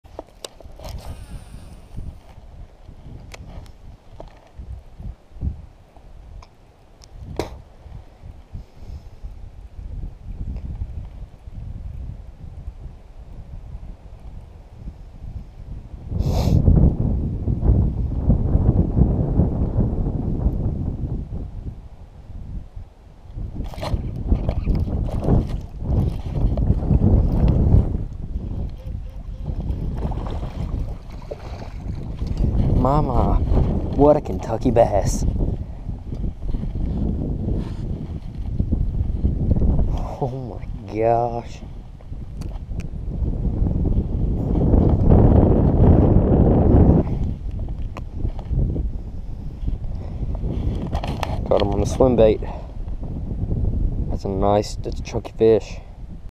LOST FOOTAGE: Nice Bass on Swimbait
footage from about 3 weeks ago and caught it on bruiser swim bait.